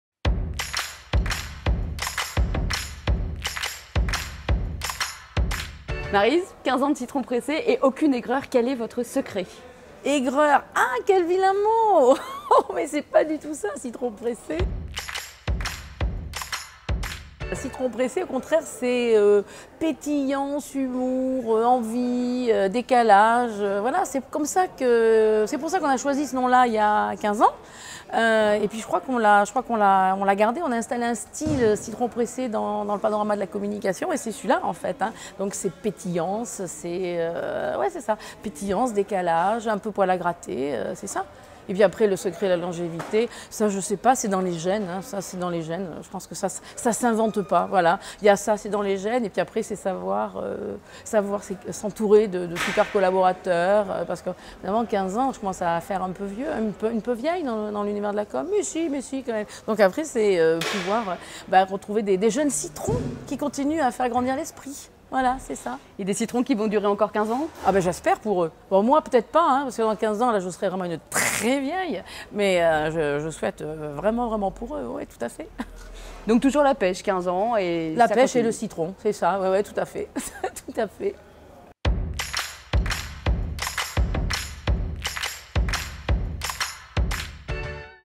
Interview de Maryse Bernard
Interview de Maryse Bernard, directrice et co-fondatrice de l'agence de communication Citron Pressé, rencontrée à l'occasion des 15 ans de l'agence